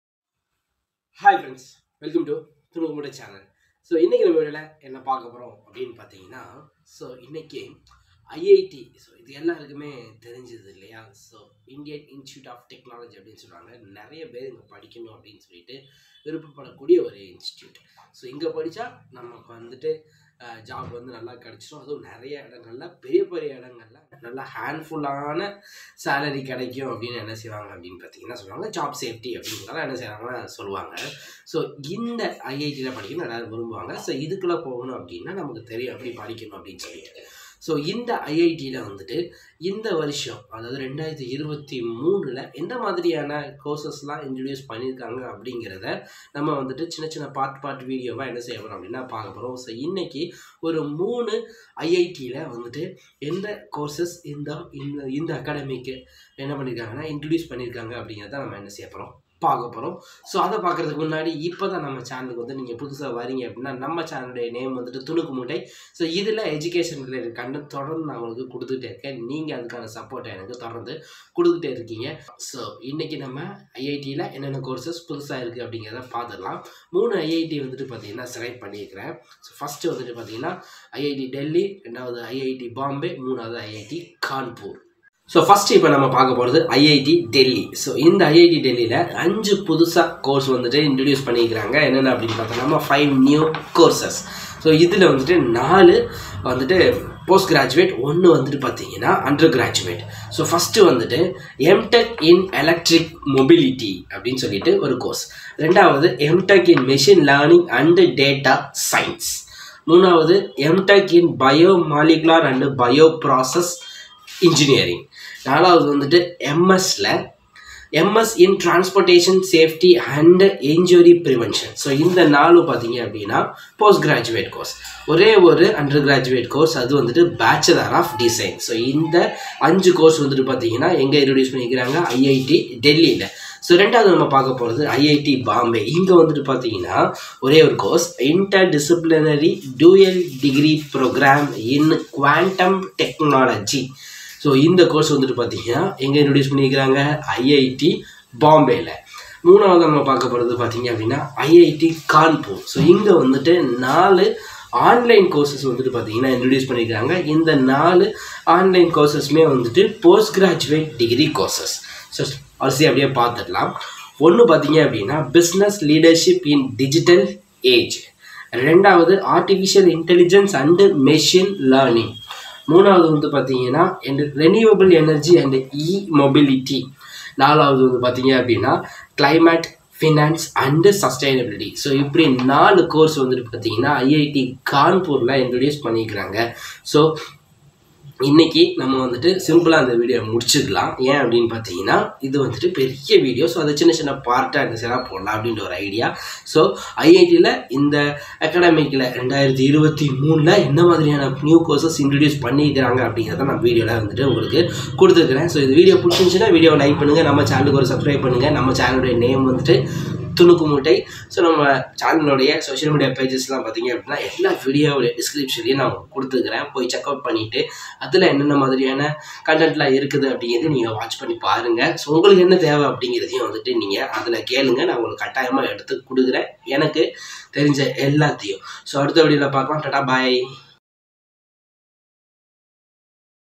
New Courses in IIT in 2023
This video gives you details about the New course of introduced in the IIT campus in all over India.
It is very useful to the students who search for the new courses.